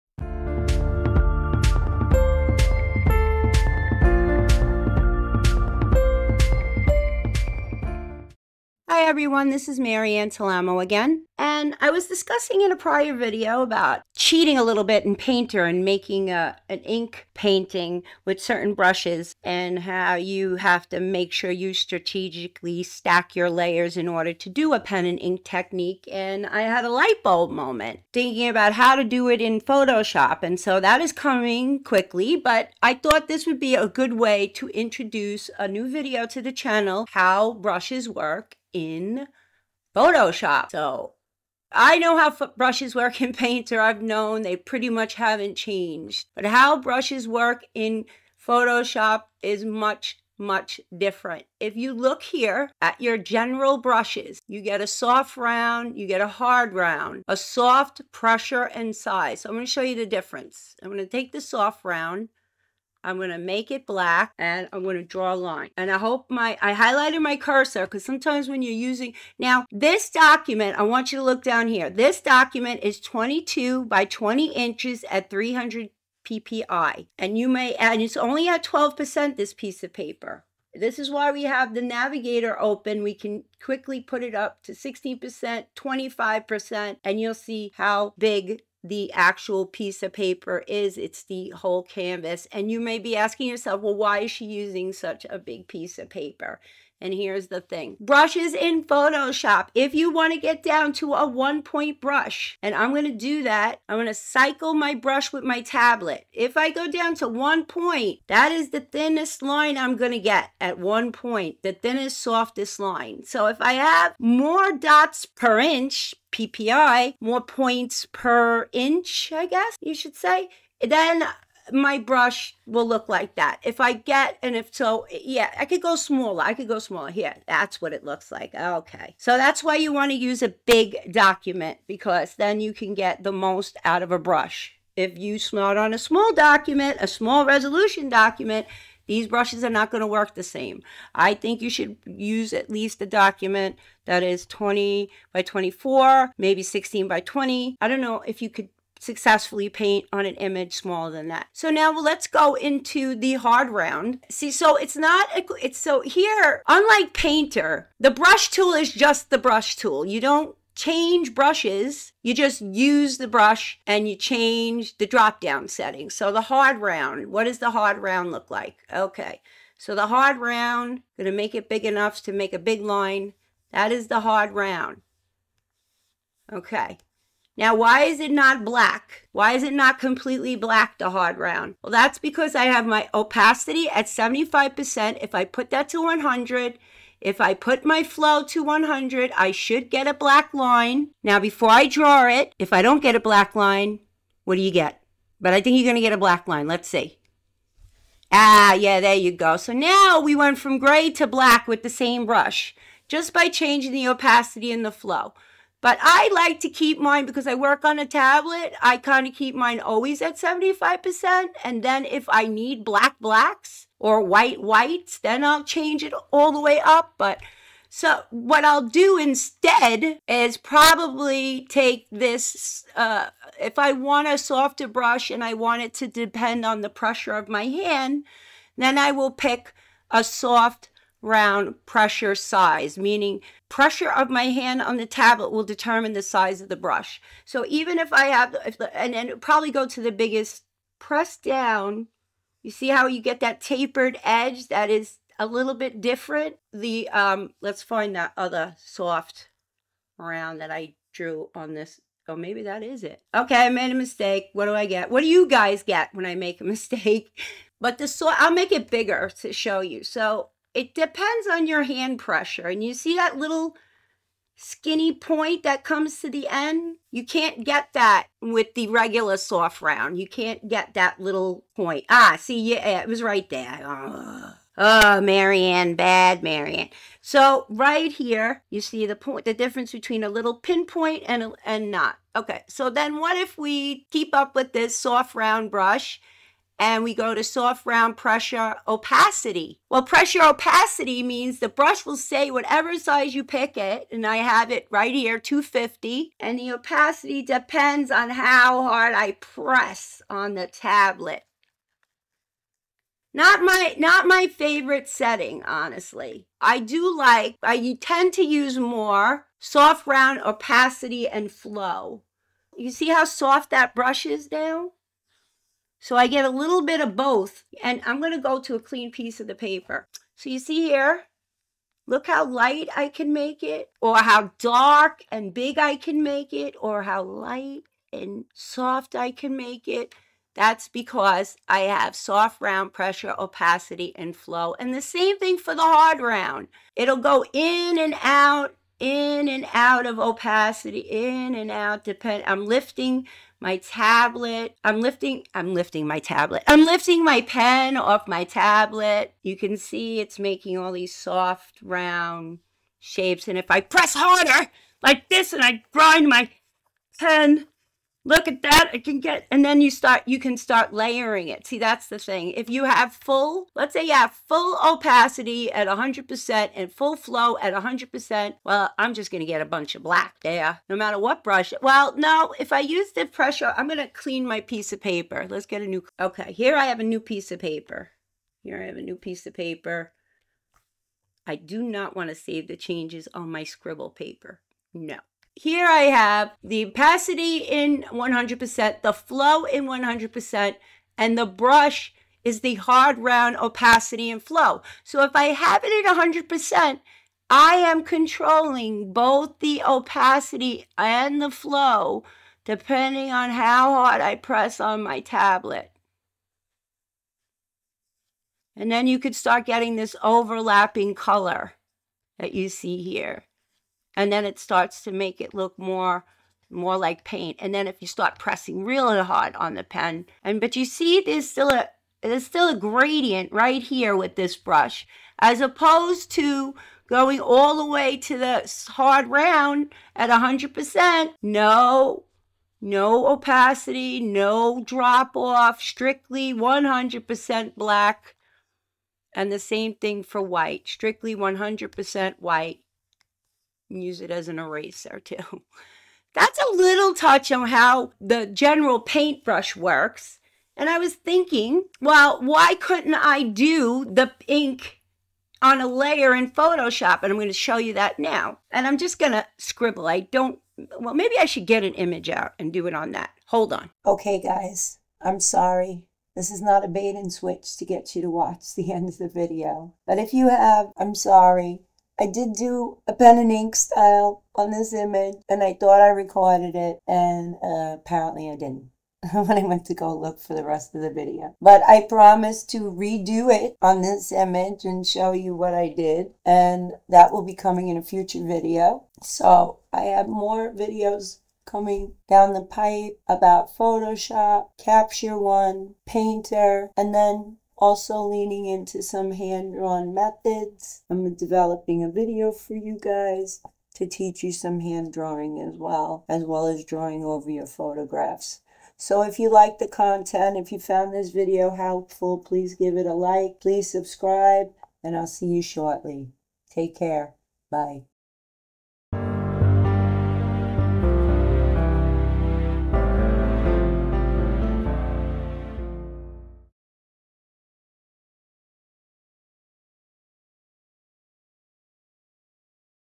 How The Brush Tool Works In Photoshop
Join me, especially if you enjoy your T's pronounced like D's, as I touch on how the Brush Tool in Photoshop differs from the brushes you find in Corel Painter. I will discuss what effect your opacity and flow will have on your application of "paint" or color. I will lead you through why there are so many choices in the dropdown menu of the Brush tool too!